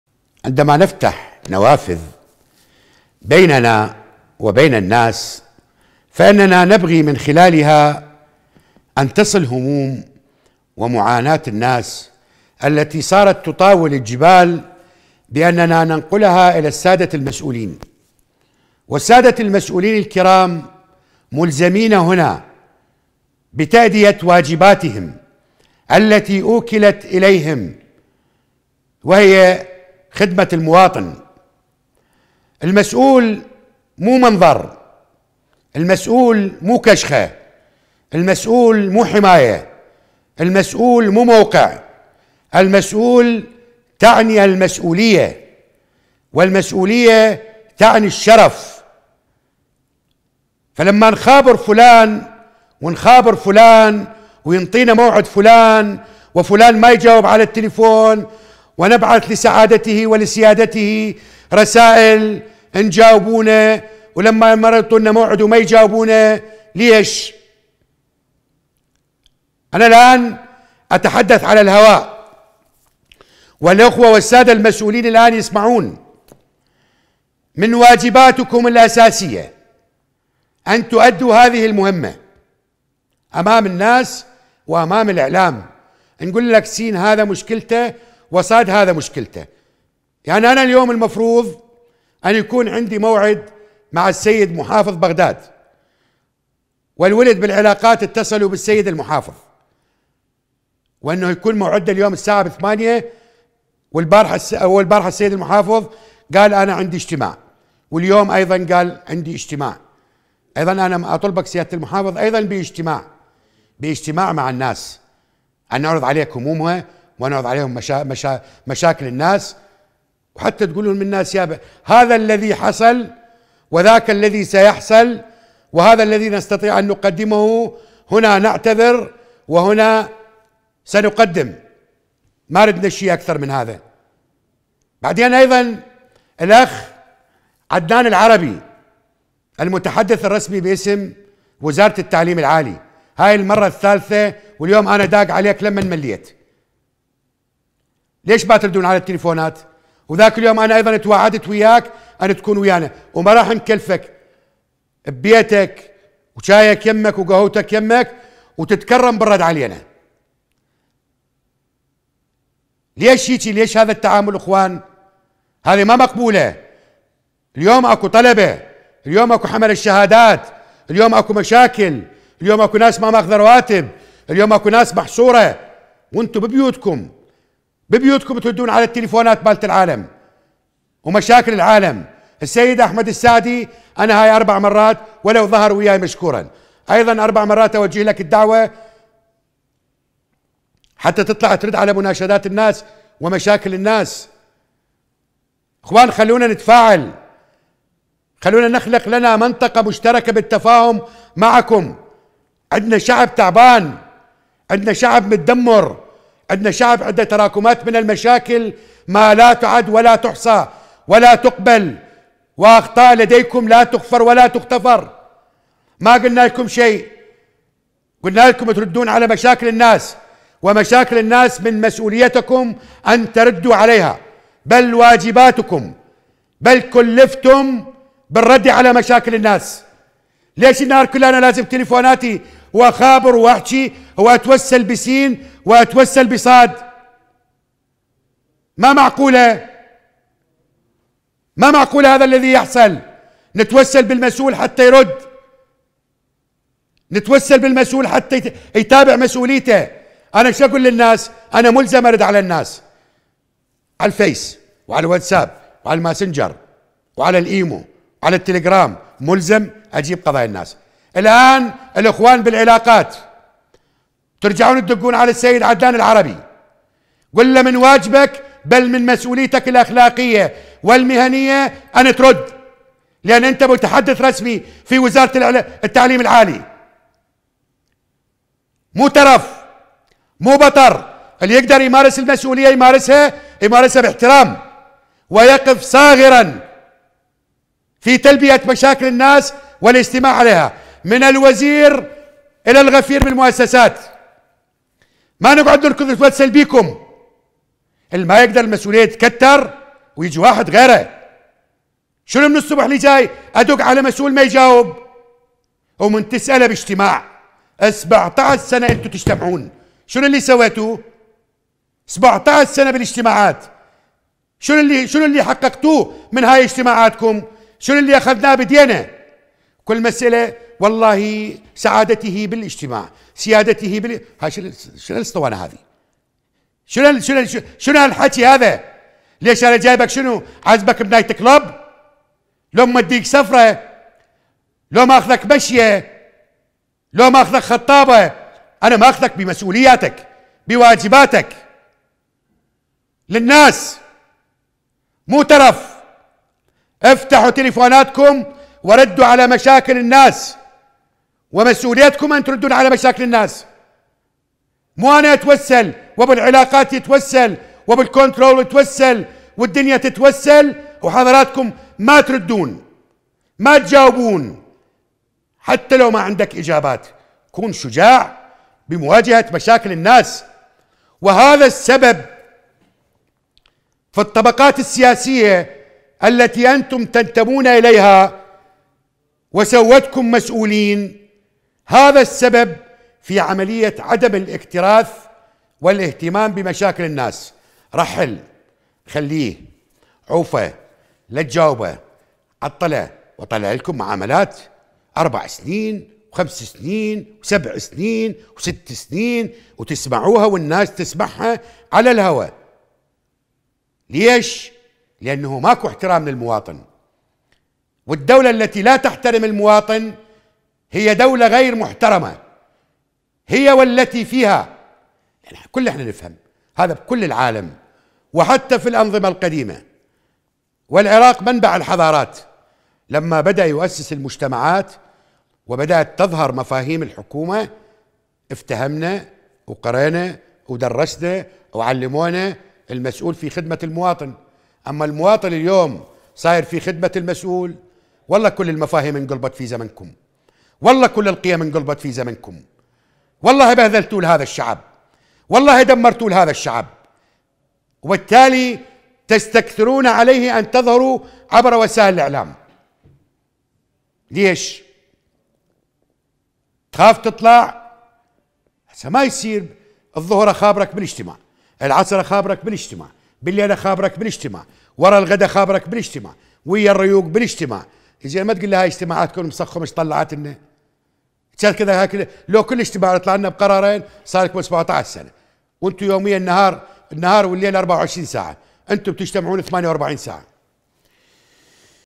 نجم الربيعي: الدولة التي لا تحترم المواطن هي دولة غير محترمة
الضيوف:
- النائب غازي الكعود
- النائب د. غايب العميري
- النائب علي جبار
لمشاهدة الحلقة كاملة: